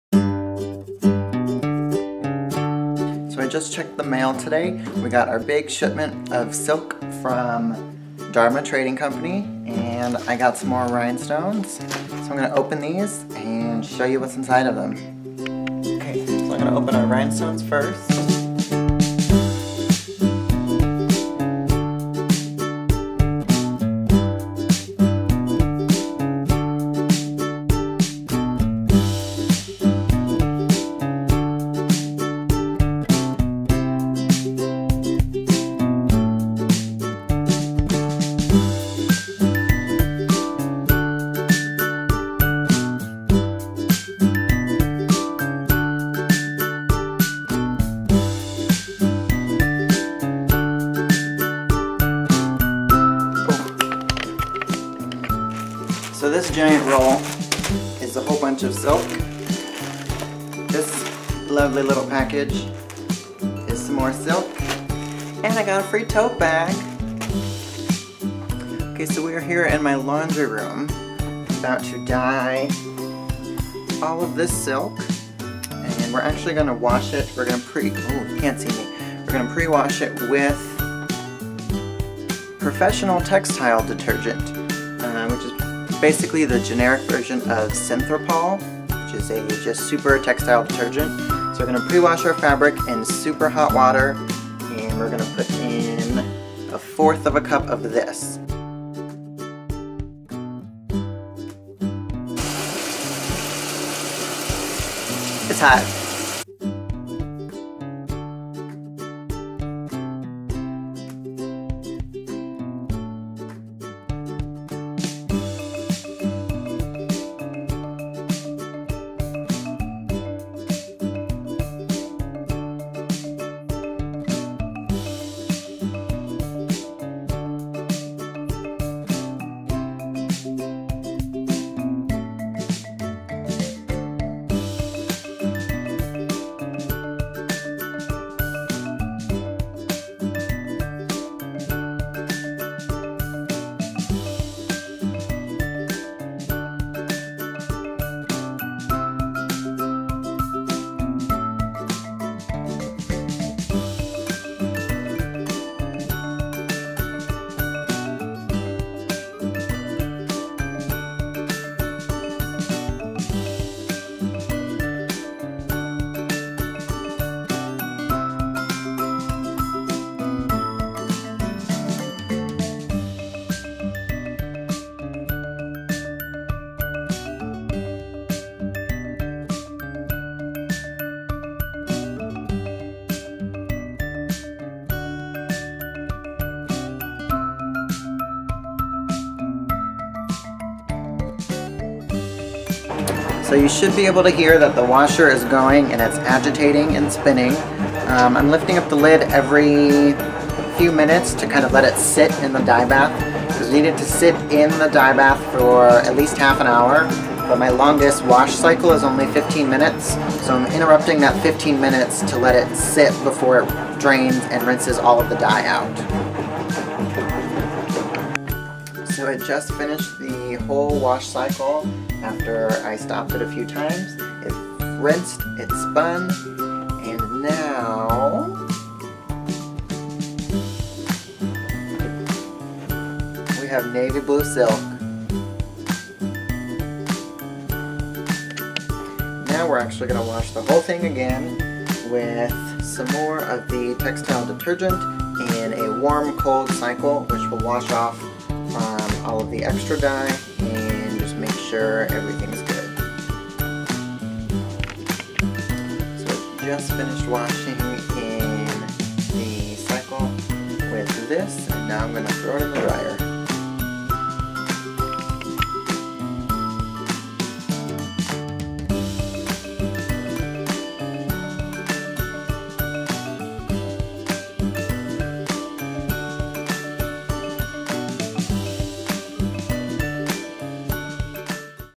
How I use Dharma Trading Co. Acid Dye ( Mini Tutorial)
This is a simple video on how I used Dharma Acid dyes to dye some raw silk, also purchased from Dharma. This video is not sponsored, I just love their products so much I decided to share.